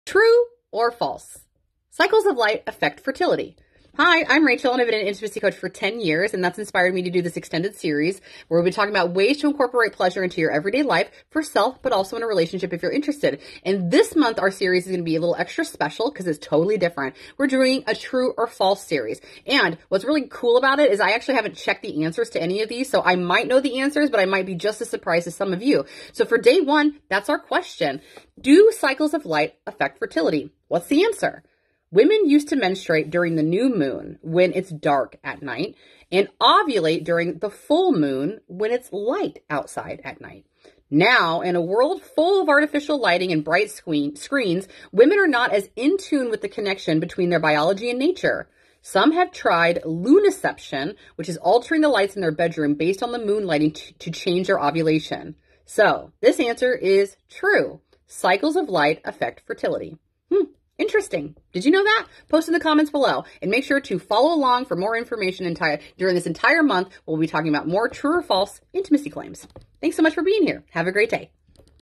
Can Light Affect Fertility?
True or False: Light Affects Fertility

Hi, I'm Rachel and I've been an Intimacy Coach for 10 years. This short series brings a beginner into the world of understanding some of the basics around sex, intimacy, & bodies! Tune into this video to learn a little bit more about what I do!